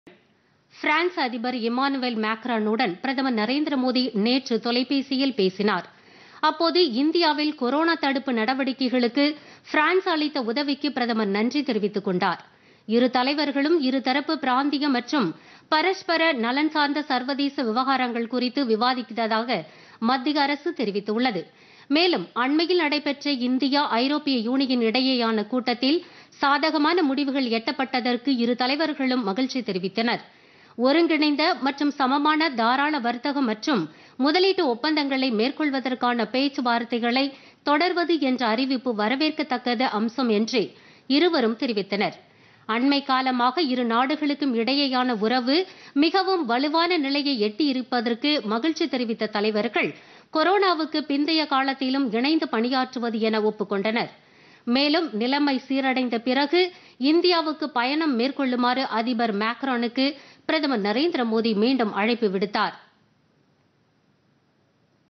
பிரான்ஸ் அதிபருடன் பிரதமர் பேச்சு
பிரான்ஸ் அதிபர் இமானுவேல் மேக்ரானுடன், பிரதமர் நரேந்திர மோதி தொலைபேசியில் பேச்சு - கொரோனா தடுப்பு நடவடிக்கைகளுக்கு உதவியதற்கு நன்றி தெரிவித்தார்.